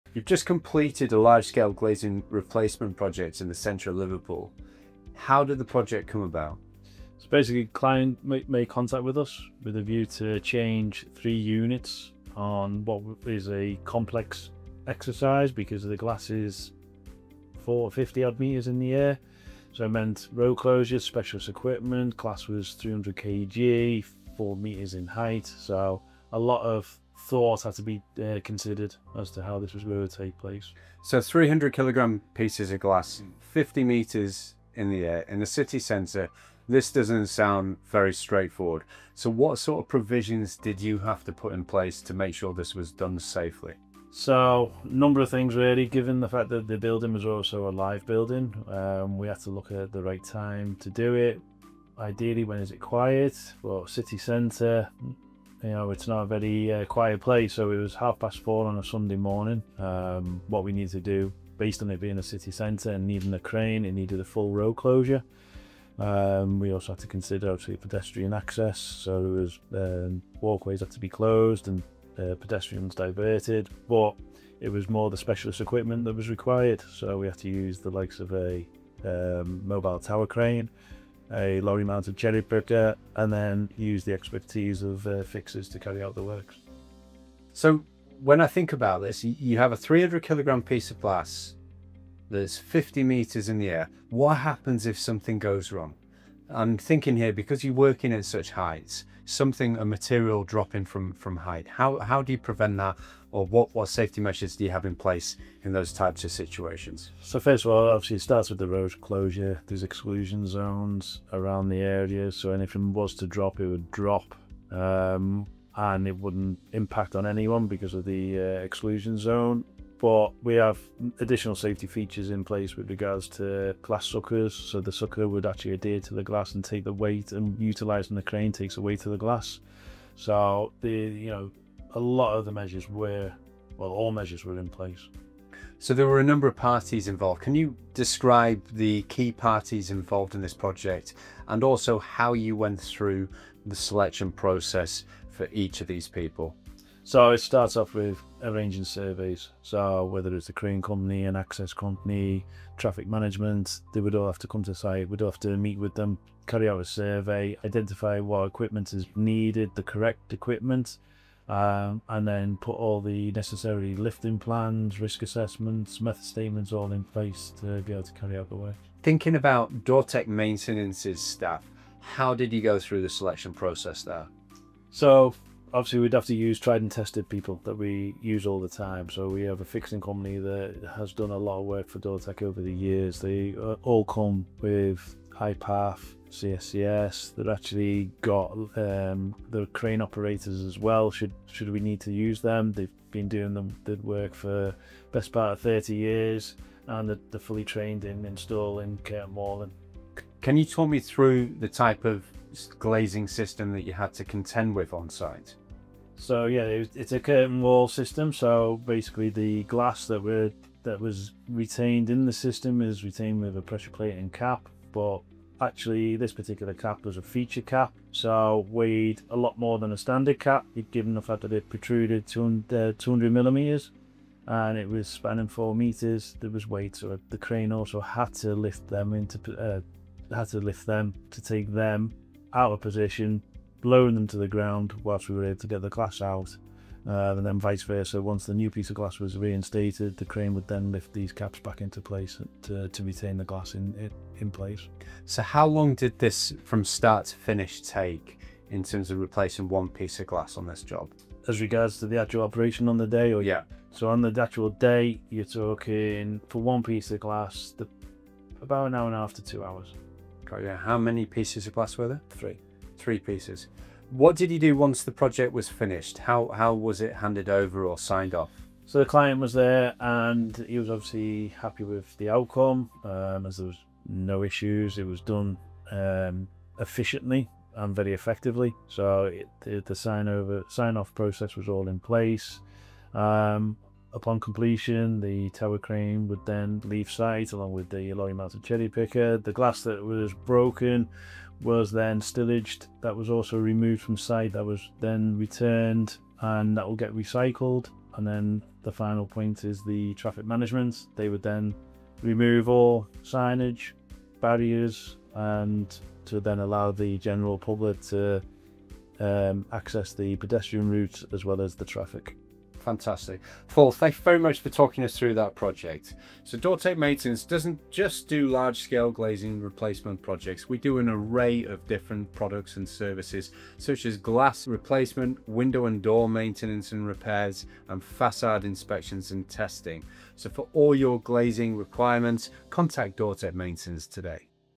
Giant 4M Glass Installed 50M High! | Liverpool City Centre Mega Glazing Project
Ever wondered how massive glass panels are replaced 50 metres above the ground?

In this exclusive podcast interview, we dive deep into one of the city’s most ambitious glazing projects — a 4-metre tall glass replacement at sky-high levels. 🚧

Our guest shares the challenges, risks, and behind-the-scenes stories of working on a landmark building in Liverpool city centre. From engineering precision to the human effort behind the glass, this conversation uncovers the true scale of modern construction and glazing.

How giant glass panels are safely replaced at extreme heights

The unique challenges of glazing projects in busy city centres

What it takes to deliver world-class results in the glazing industry

If you’re into architecture, construction, engineering, or Liverpool landmarks, this video is a must-watch! 🎙️✨